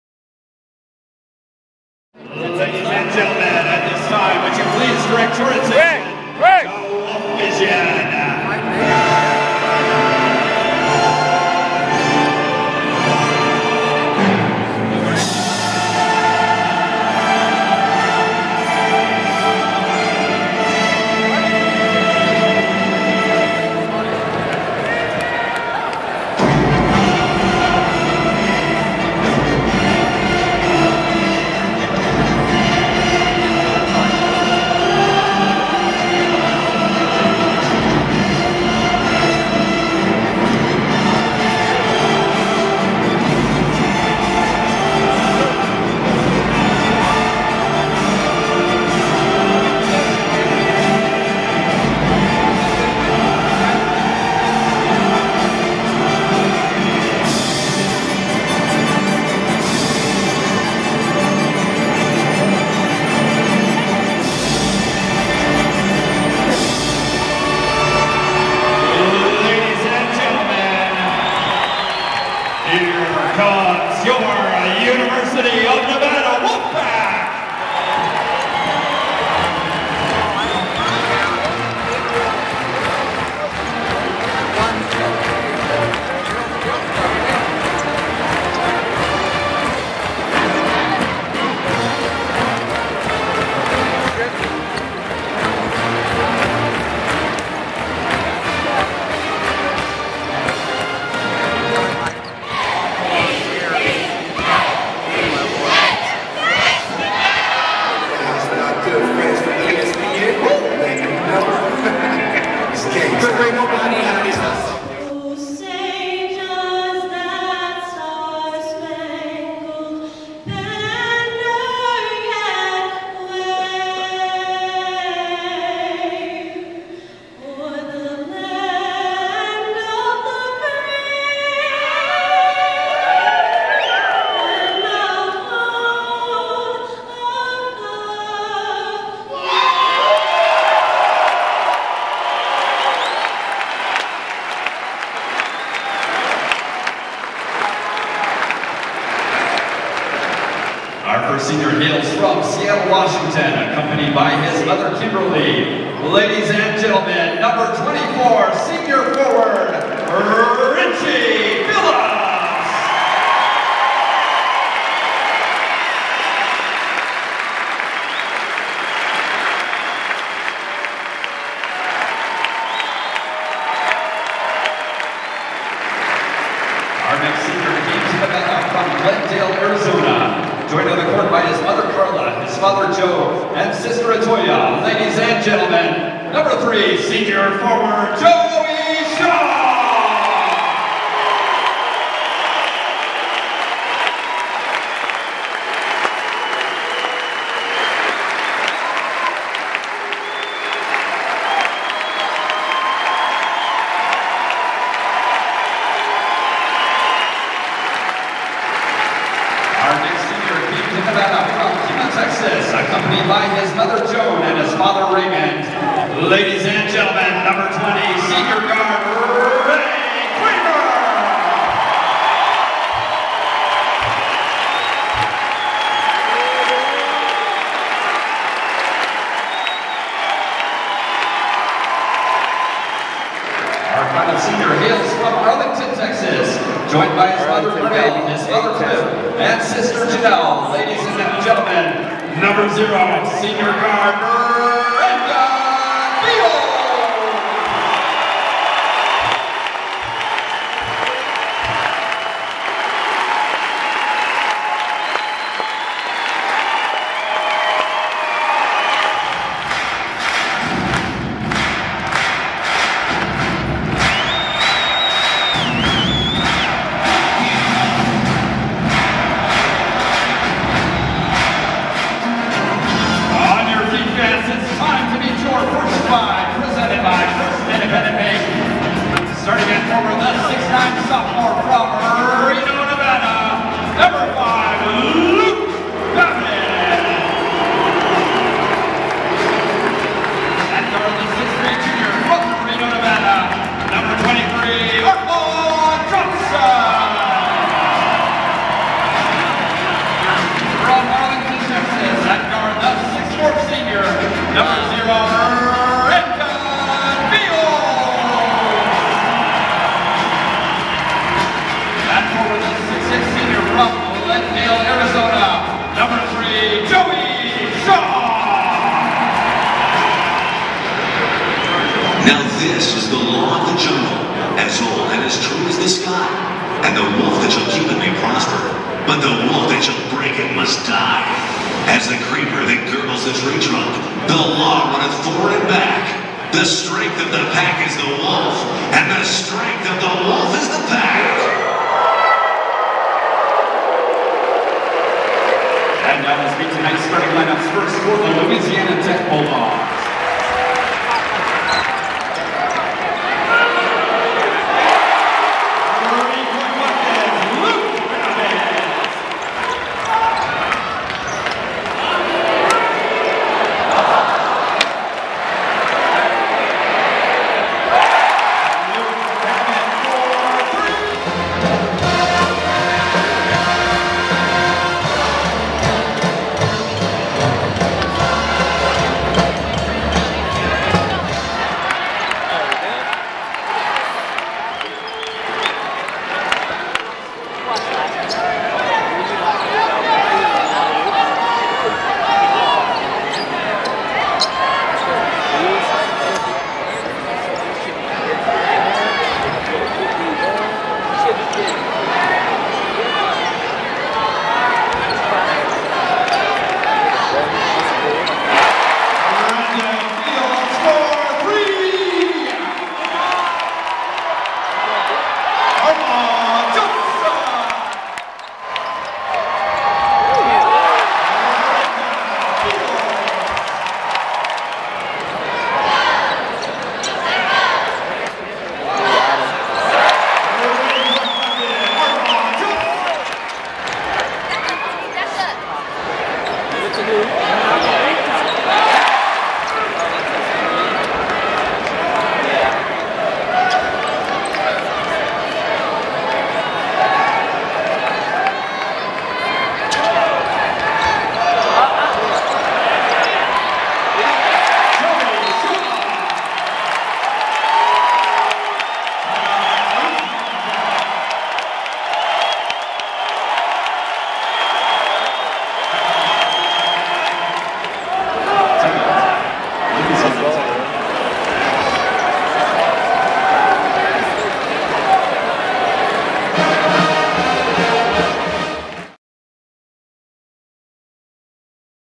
Senior Night University of Nevada, Reno Basketball
The University of Nevada, Reno celebrates Senior Night for the 2009 Basketball season. Lets go Pack!